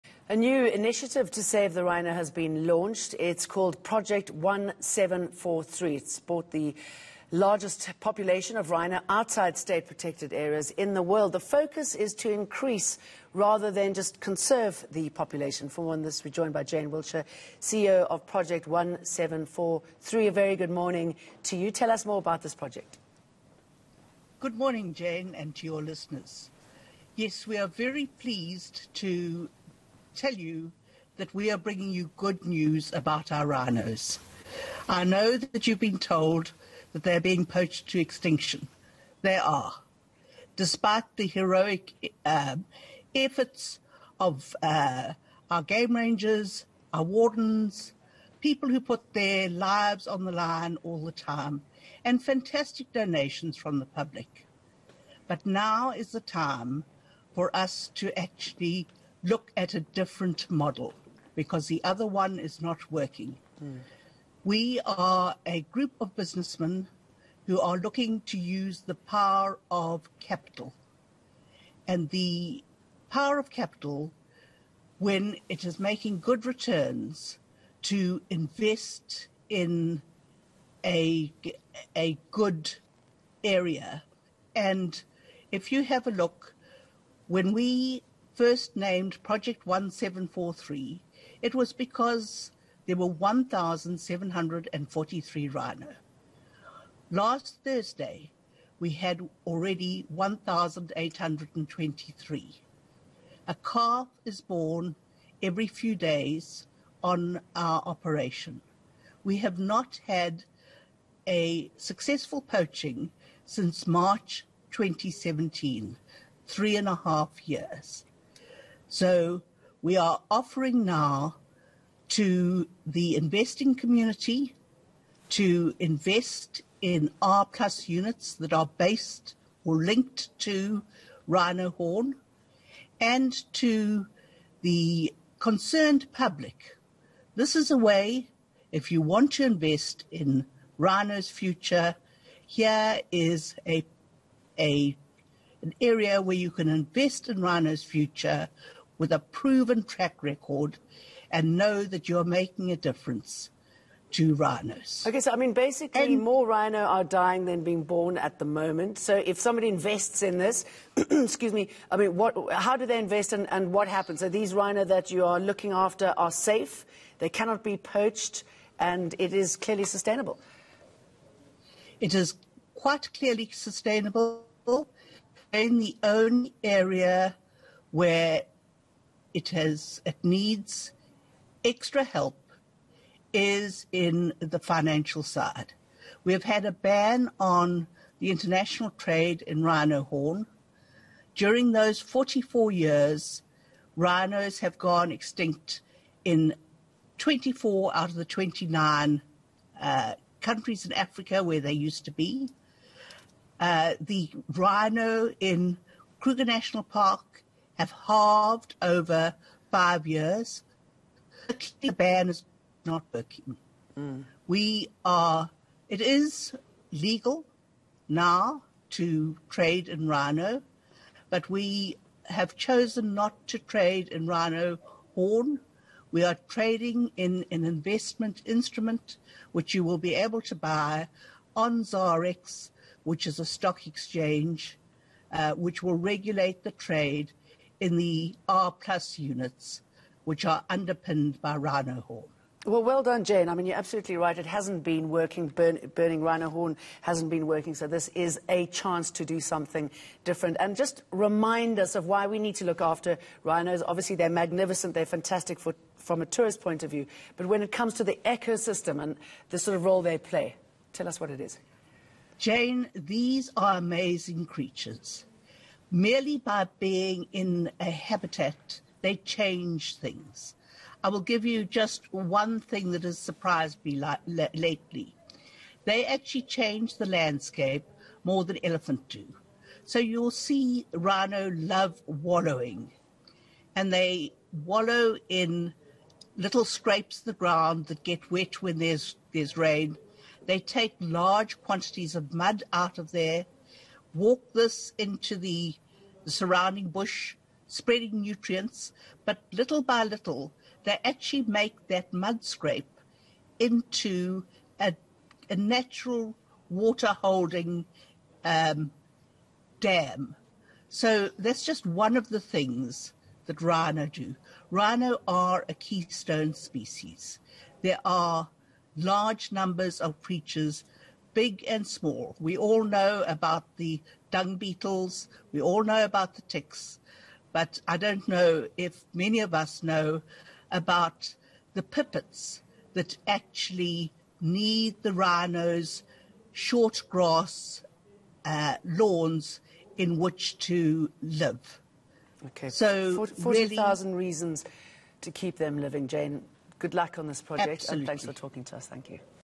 Save the rhino | Population increases by 6% every year
Project 1743 has bought the largest population of Rhino, outside state-protected areas, in the world. CEO of Project 1743 Jane Wiltshire spoke with eNCA's Jane Dutton. Courtesy of DStv403